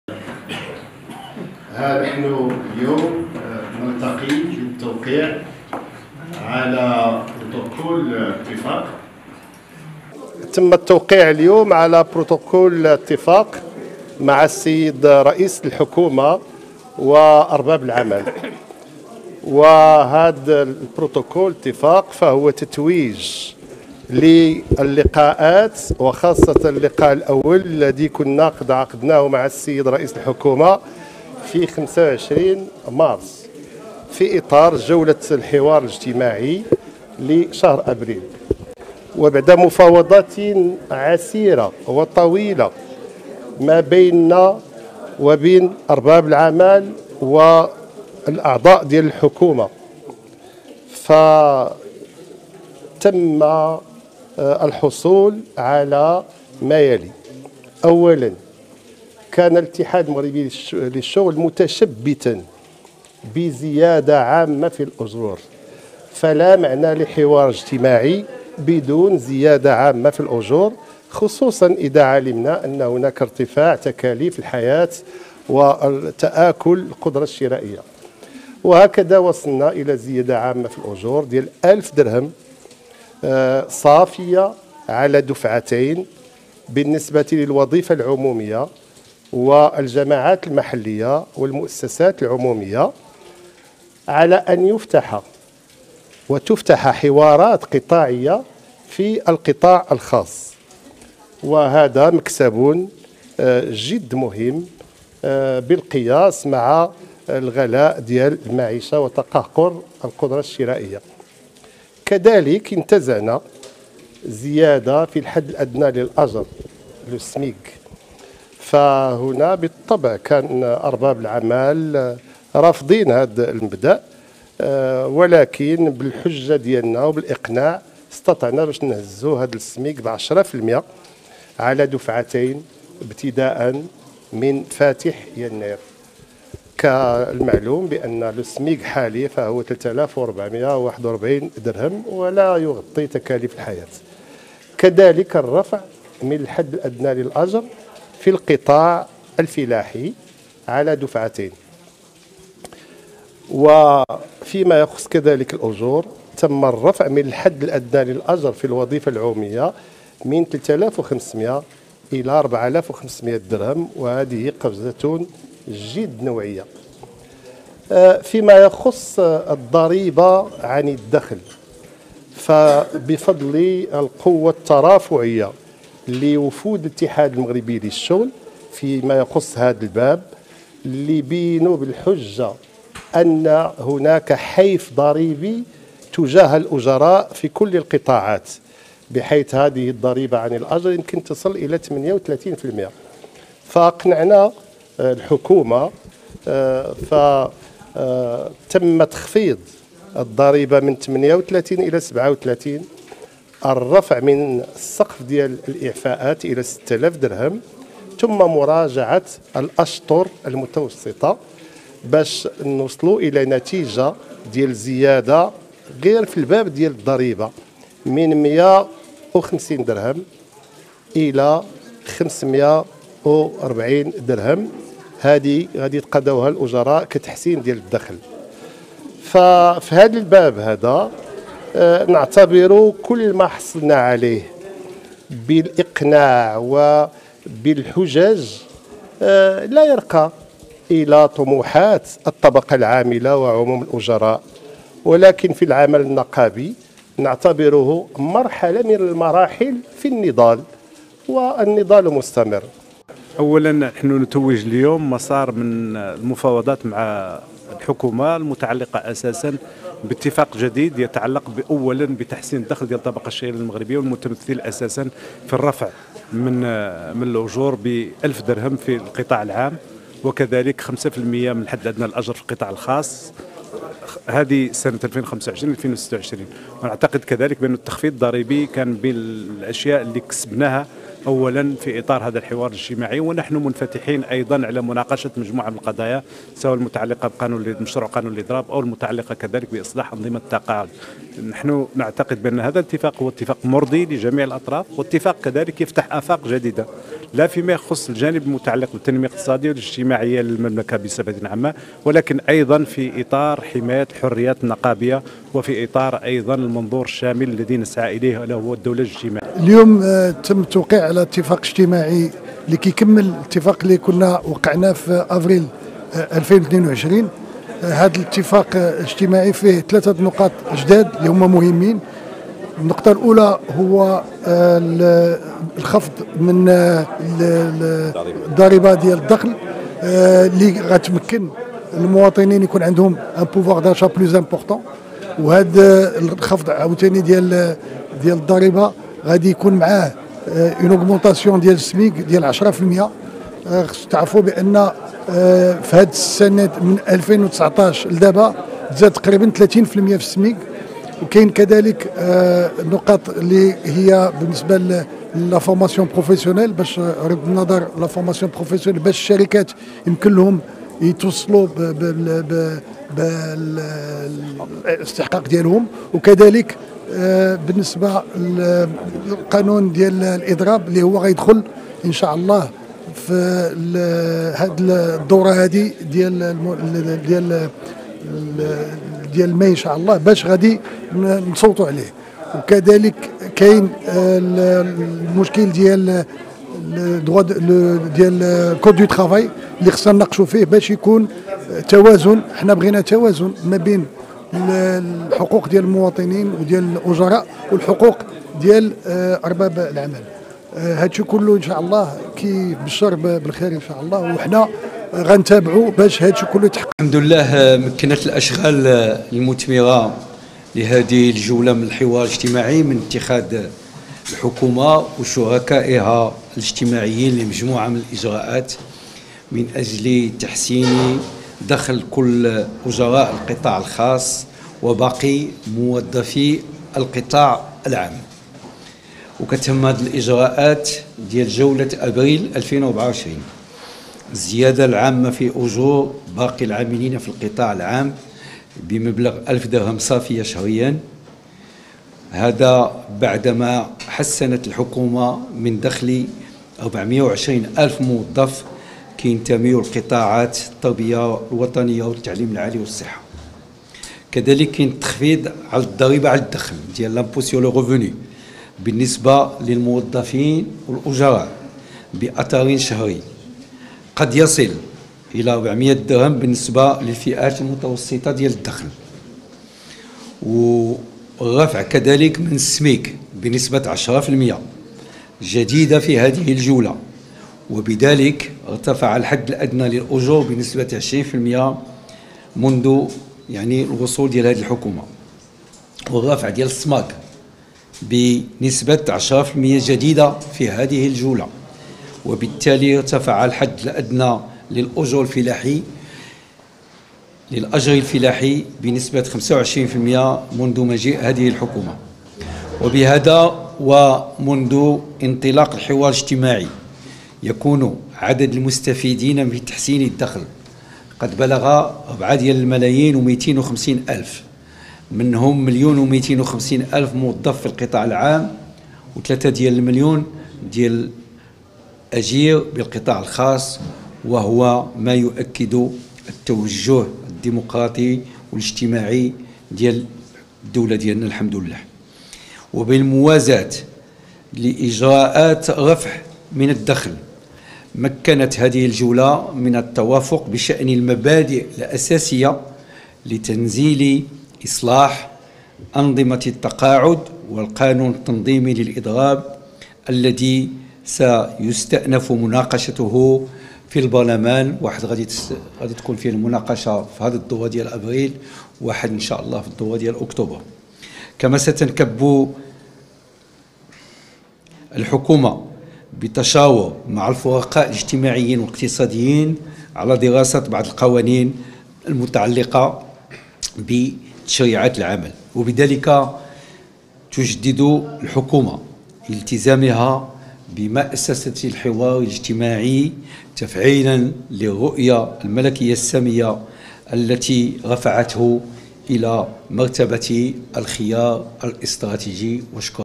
الحكومة تكشف عن قرار الزيادة في الأجور/ معاشات التقاعد🤔؟؟ موخاريق يوضح التفاصيل 10 فالمائة فالسميك
الزيادة في الأجور ومعاشات التقاعد و10 في المائة للسميك 🚨 موخاريق يكشف عن خلاصة الحوار مع الحكومة 🔥💵💵